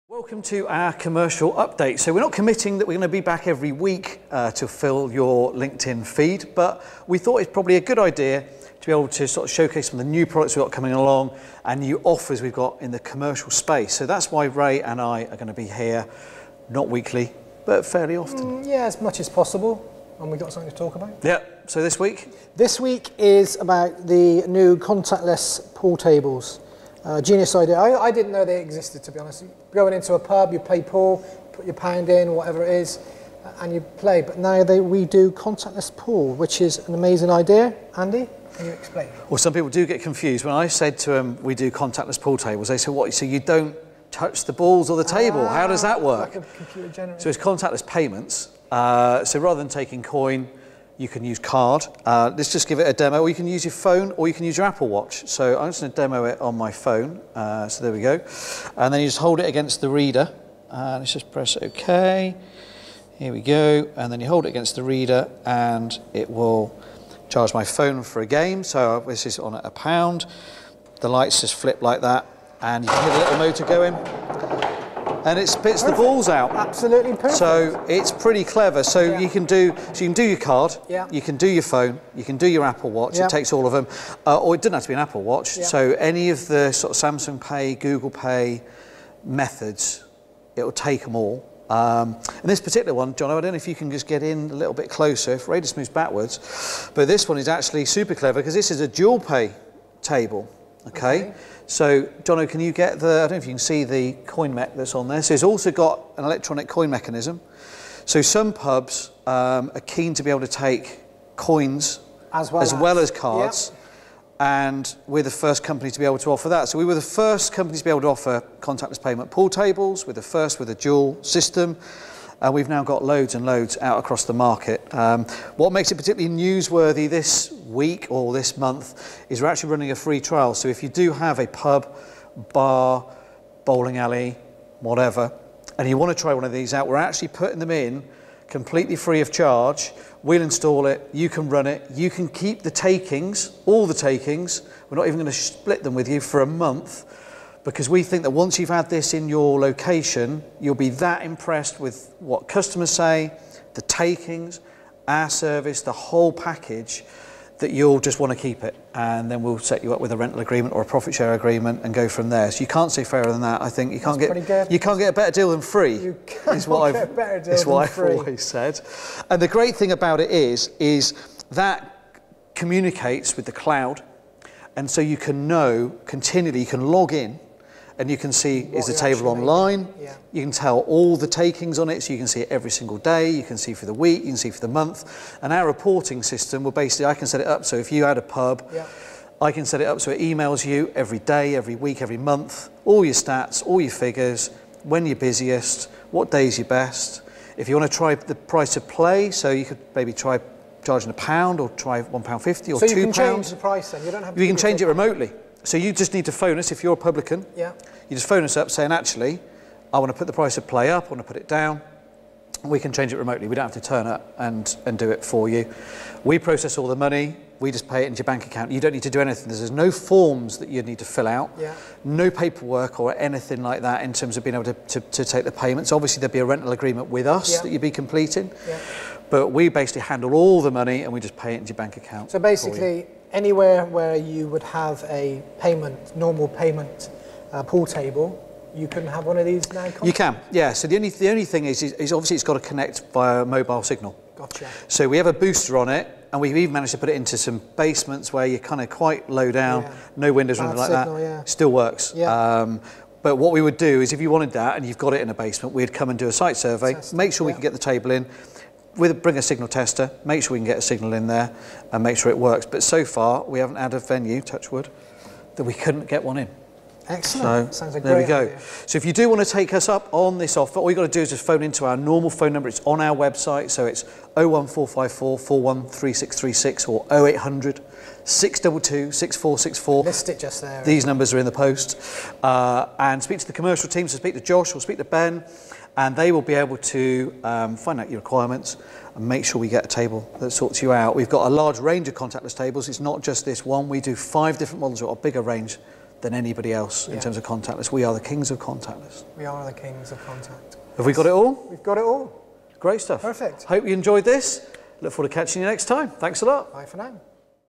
Commercial News - Contactless Payment Pool Tables
Contactless Pool Tables
Market leading range of contactless pool tables for the commercial market. Process payments securely by card or mobile phone. No more counting cash, jammed coin mechs and customers queuing for change. The future is cashless.

Looking for this PRODUCT? Available with award winning service from The UK's largest Games Room Retailer. Pay now or buy on finance!